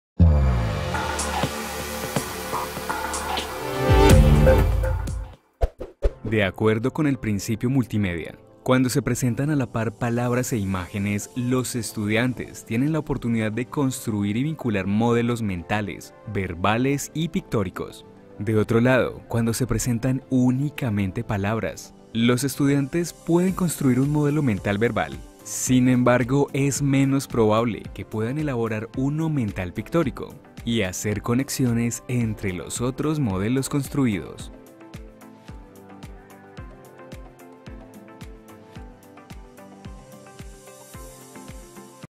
Principio multimedia
De acuerdo con el principio multimedia, cuando se presentan a la par palabras e imágenes, los estudiantes tienen la oportunidad de construir y vincular modelos mentales, verbales y pictóricos.